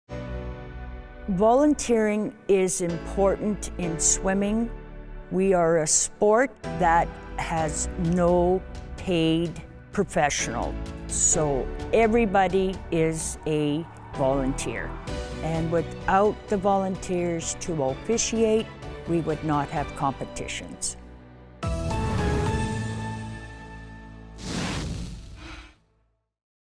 2021 Volunteer Week - Marni Caldwell
National Volunteer Week is April 18-24. This week, Sask Sport thanks the countless number of volunteers who work daily in the amateur sport system in the province. Marni Caldwell is a long-serving volunteer official in swimming.